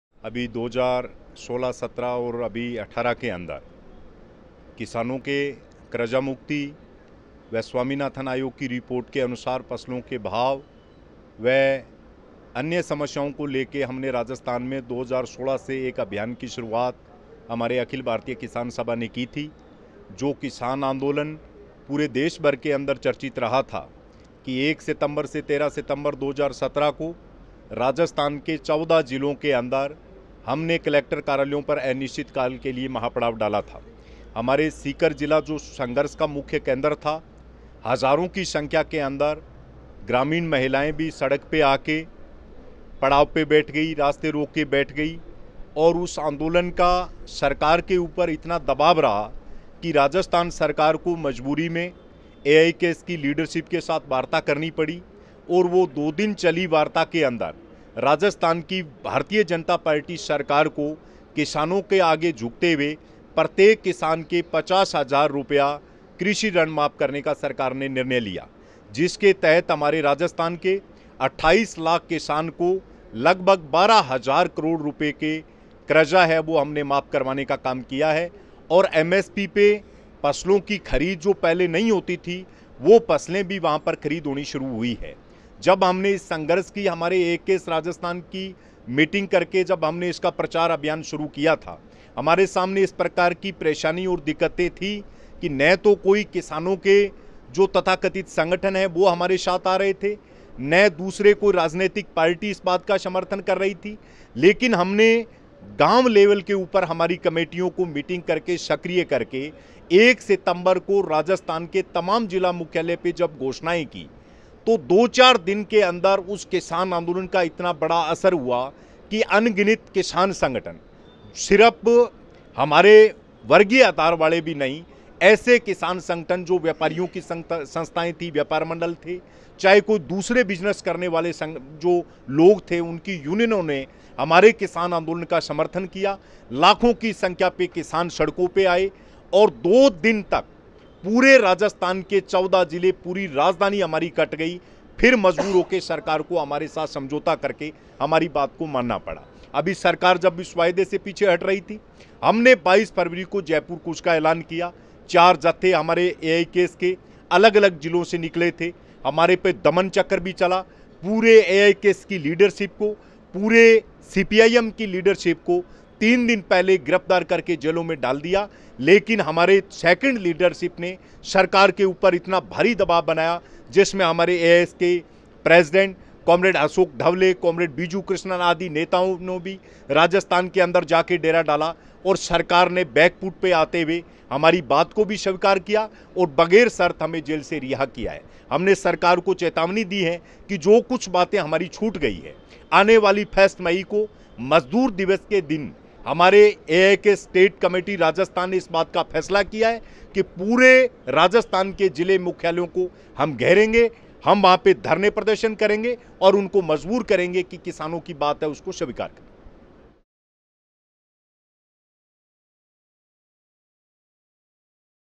Farmers Under the Leadership of AIKS All Set to Gherao District Headquarters in Rajasthan
Comrade Sheopat, Young AIKS activist talks about the farmers agitation that took place in Rajasthan. He throws light on the process of mobilisation, its impact and the way forward.